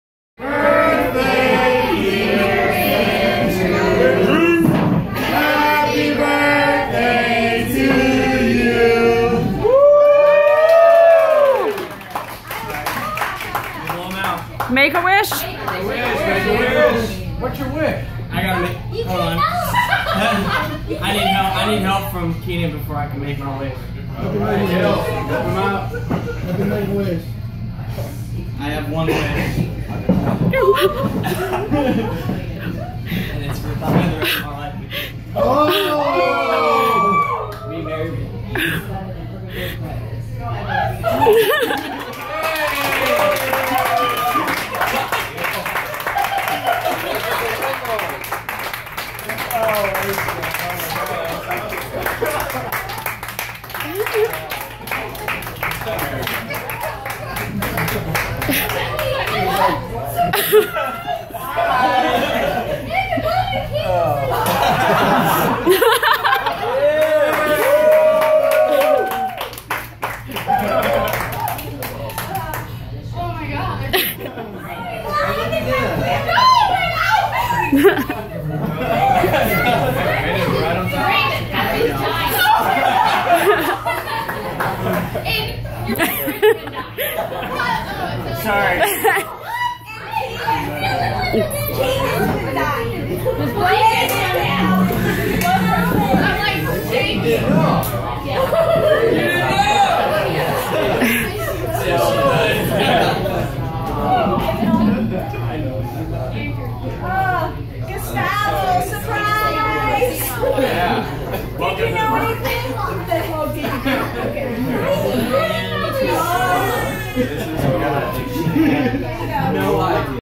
Surprise Birthday Wish Proposal
My beautiful girlfriend planned an amazing surprise birthday dinner but what she didn't realize was that I had my own surprise up my sleeve!